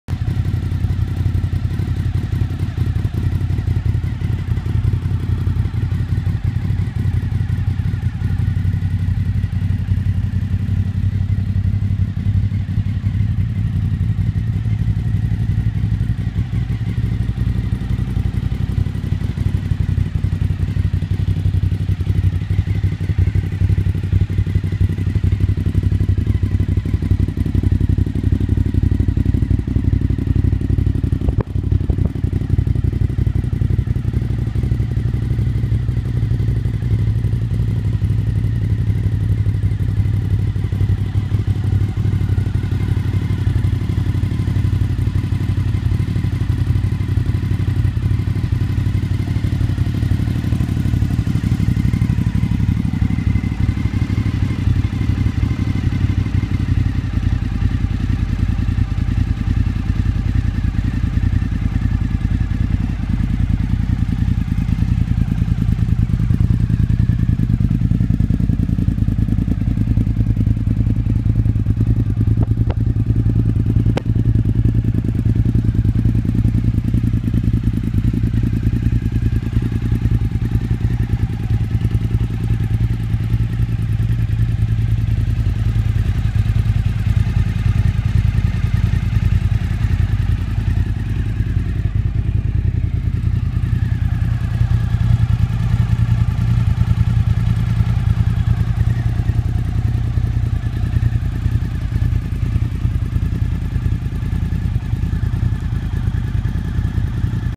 1999 TL1000R | Carbon Edition
Here's a quick walk around of my full carbon fibre TL1000R.

I can't take all the credit for building this bike as it was pretty complete before I bought it - but I did make do some mods and I'm quite proud of it. It weighs just over 400lbs so it can take on most of the latest rockets!

Carbon farings, tank, fenders and wheels. Custom HID bi-xenon headlights and HID tail lights. Full M4 exhaust with titanium cans, painted frame, GSXR forks, radial brakes and steel rotors. Replaced the factory rotary rear suspension with Ohlins system. Too much to list.

This is a fun bike!

I just made up custom decals for the bike to be a little different. Hope you like them.